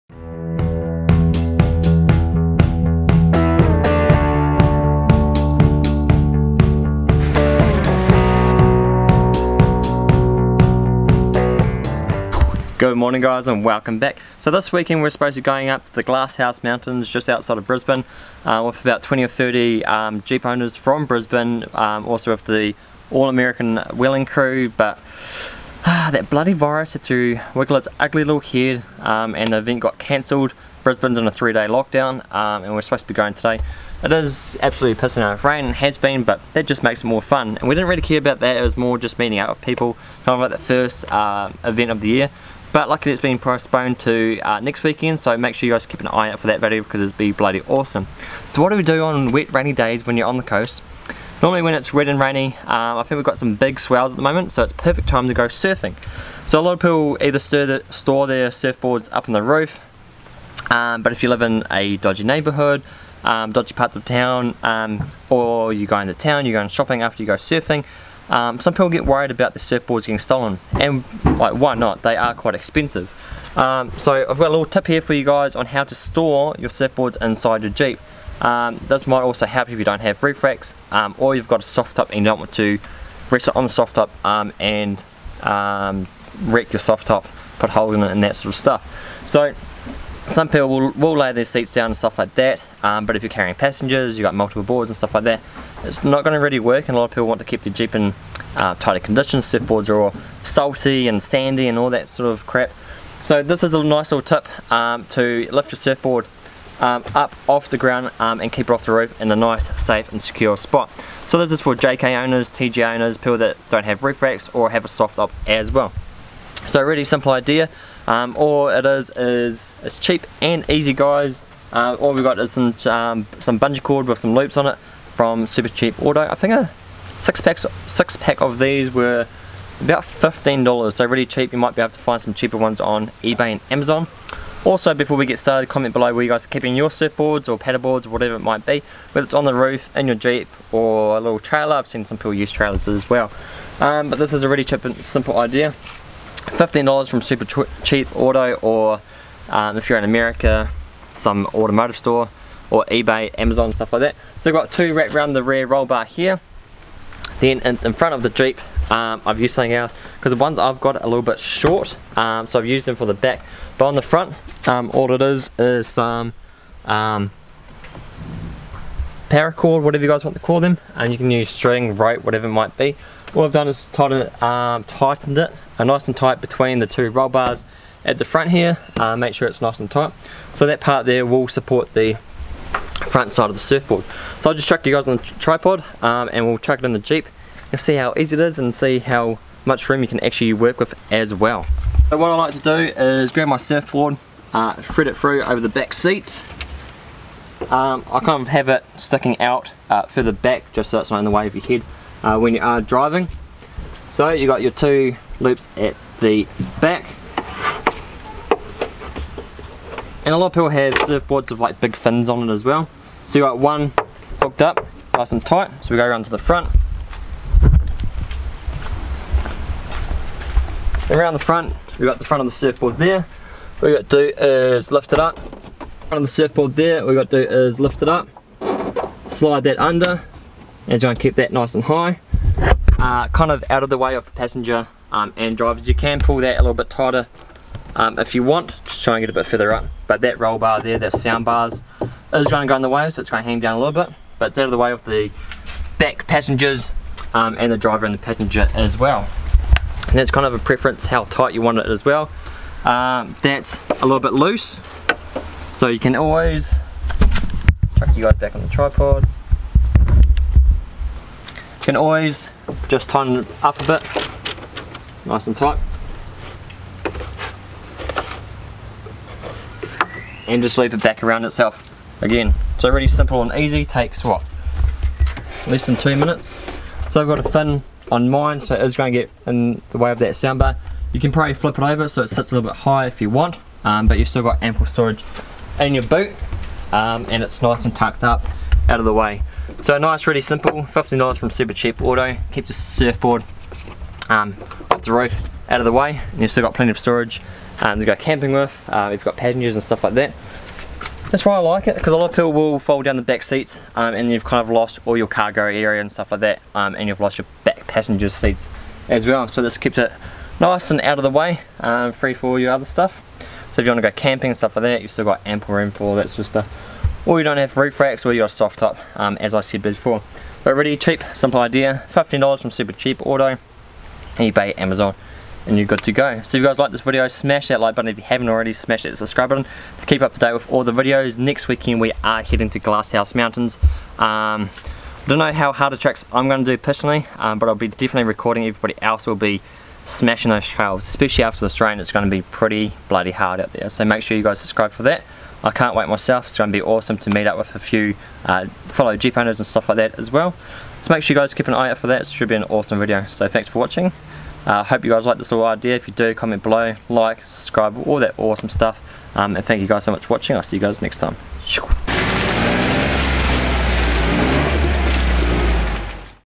JEEP HACK - SURFBOARD STORAGE
In this video we have a great little jeep hack to storage your surf board in your Jeep Wrangler.

Most people will have roof racks on their jeeps but for the people that don't or might have a soft top on their Jeep Wrangler this little hack is a great way to store your surfboard in your jeep, all your will need is a a couple of banggy cords for @supercheapautotv or any automotive shop ($15) and your have a safe way to carry your surfboard in side your jeep with out needing to fold down the seats and lose cargo space.

This jeep hack will work in pretty any Jeep Wrangler that has the roll bars- jeep jk jeep tj and lots more.